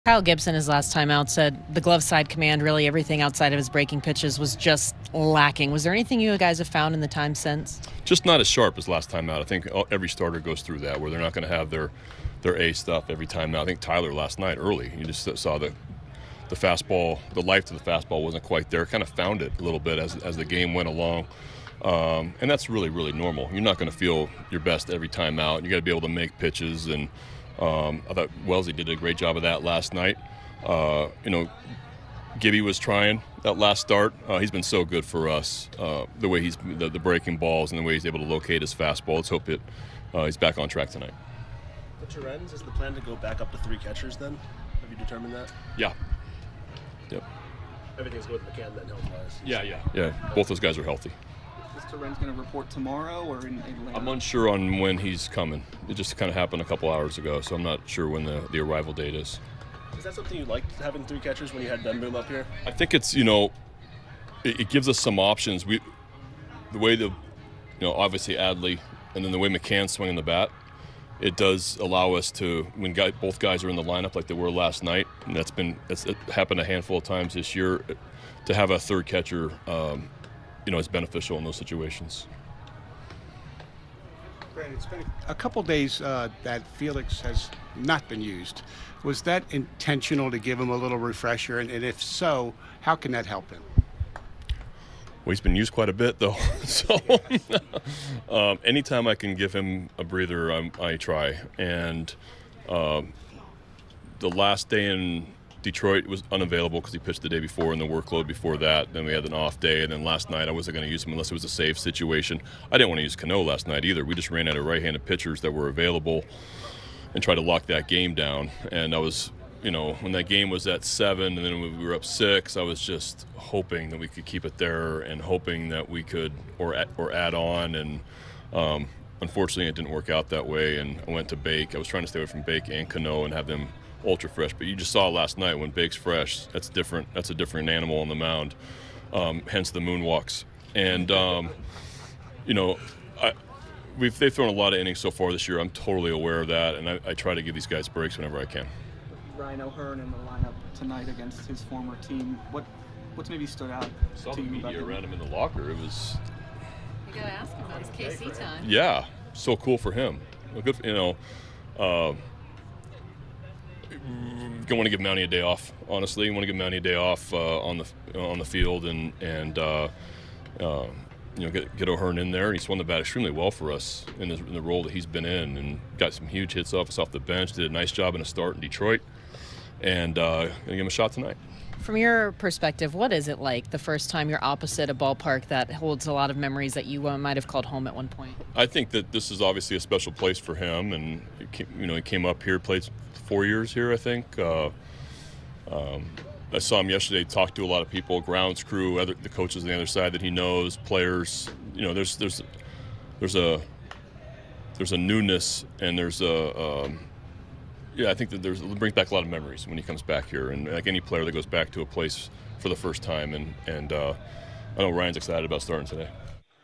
Brandon Hyde on O's plan for Luis Torrens
The Orioles acquired catcher Luis Torrens from the Cubs in exchange for cash considerations Wednesday. More